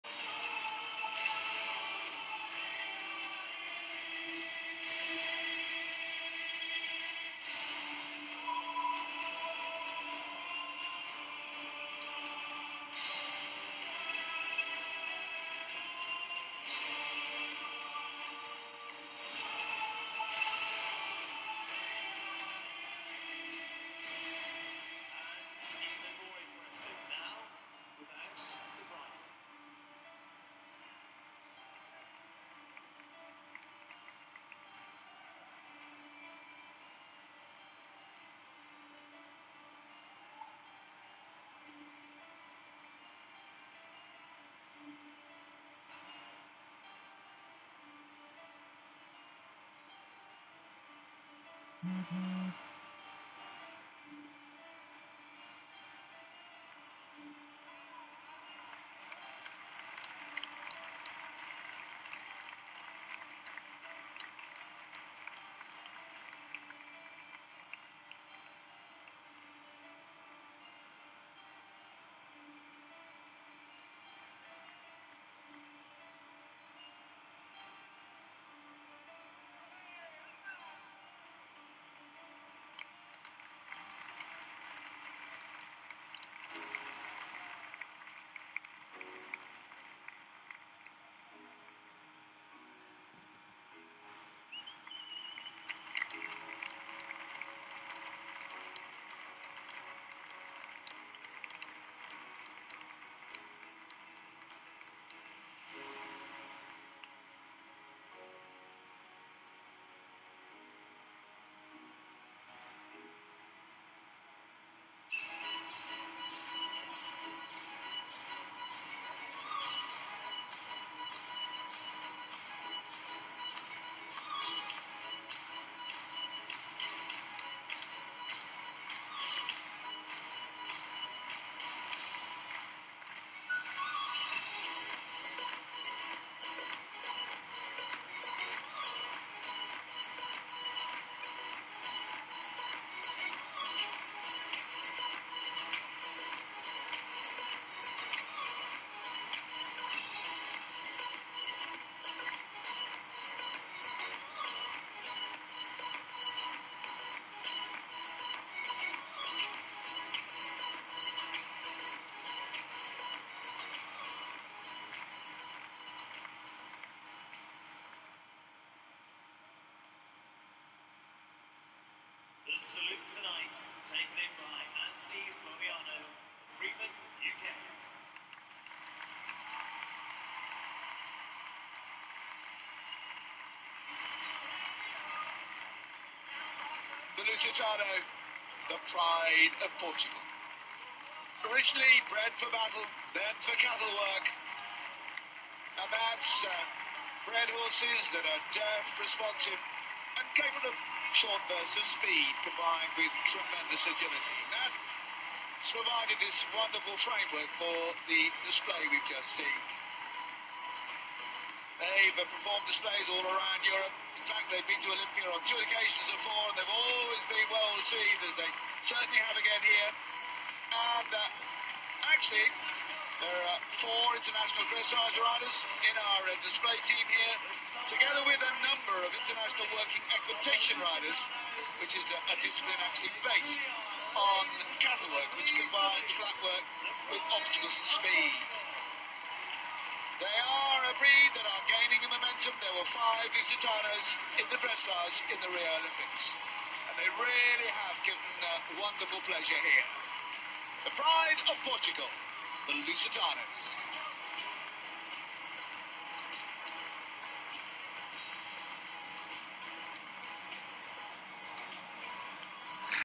Manuel Tavares Veiga | Cavalo Lusitano | Londres
Manuel Borba Veiga com Varão da Brôa em Londres no Olympia Horse Show